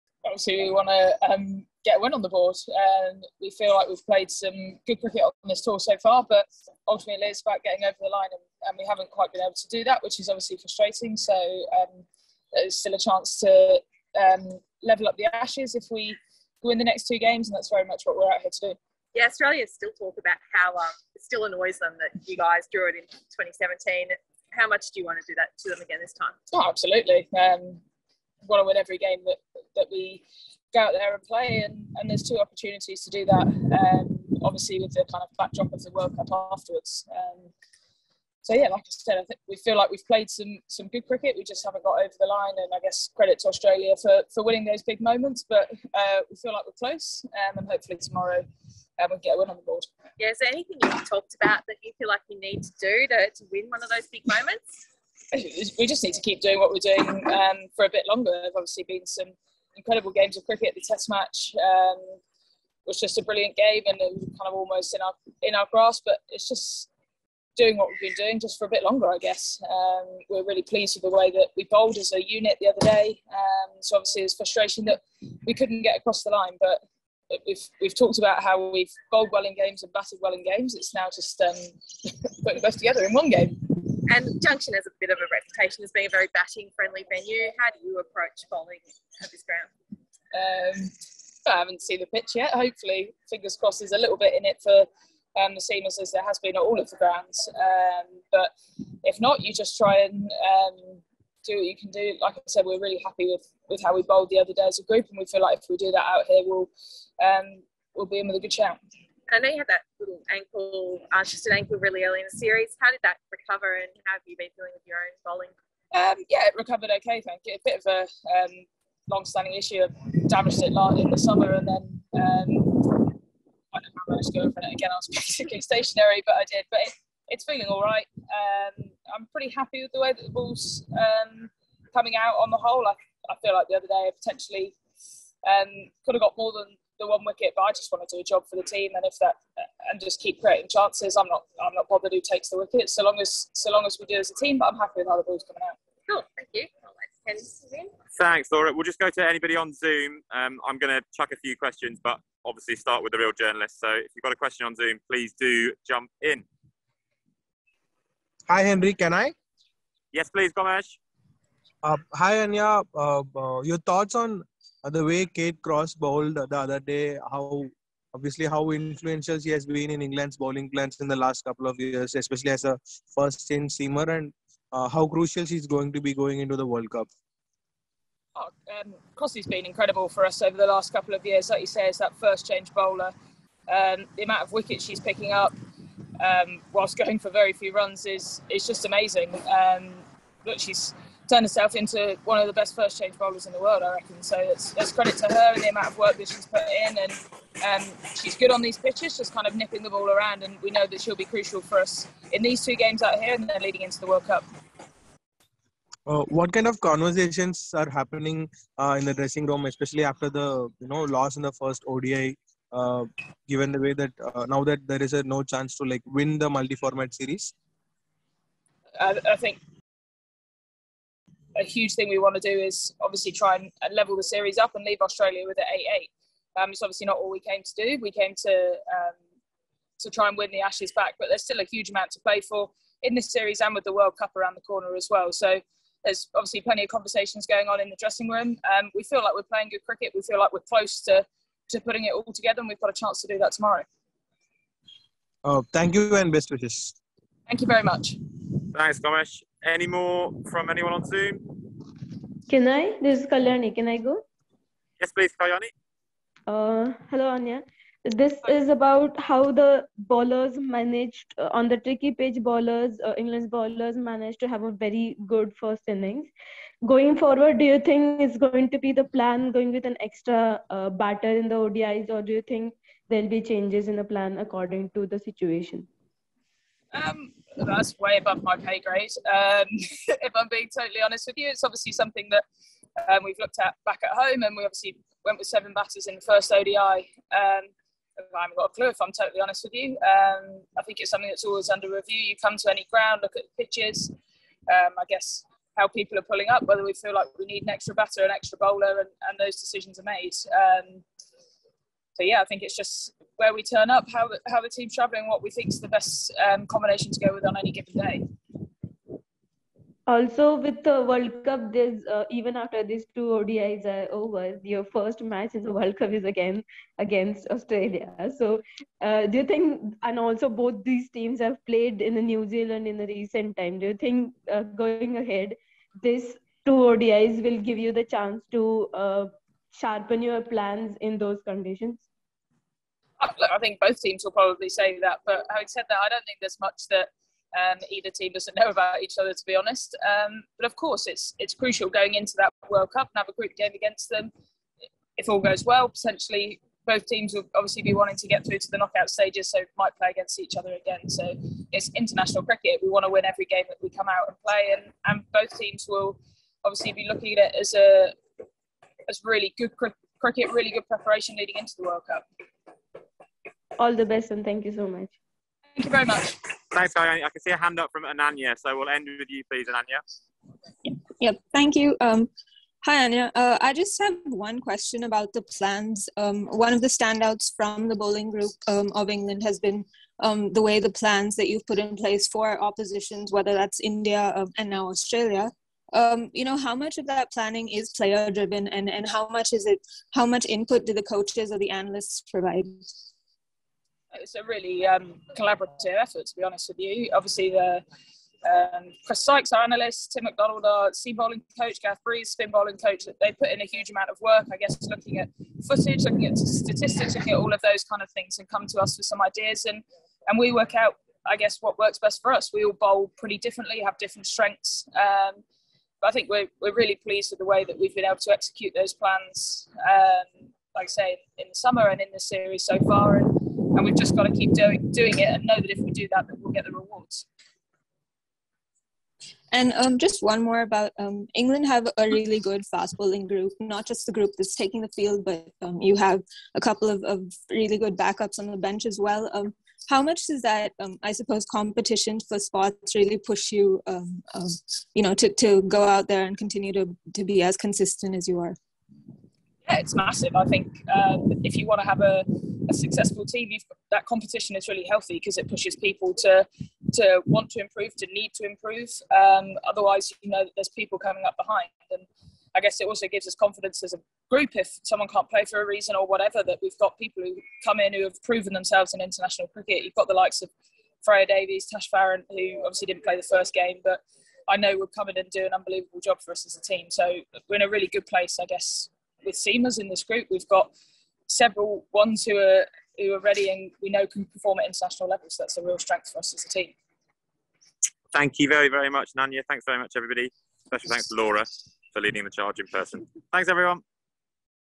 Anya Shrubsole match preview | Australia v England Women’s Ashes
Anya Shrubsole holds a pre-match press conference ahead of the second Women's Ashes ODI.